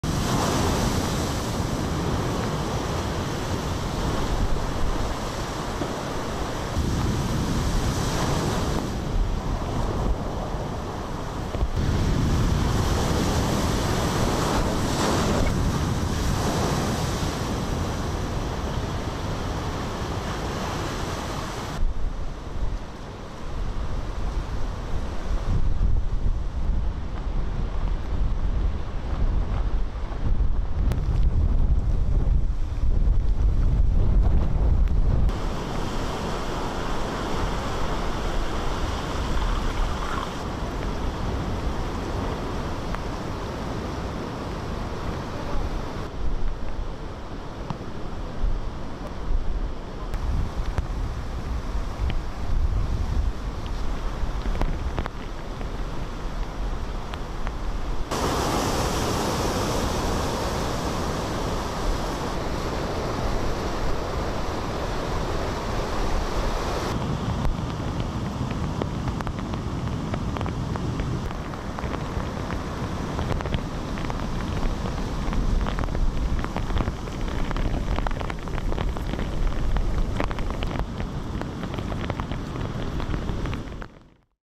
Typhoon batters South Korea
The most powerful typhoon to hit South Korea in years battered its southern region on Tuesday, dumping almost a meter (3 feet) of rain, damaging roads and power lines. (September 6)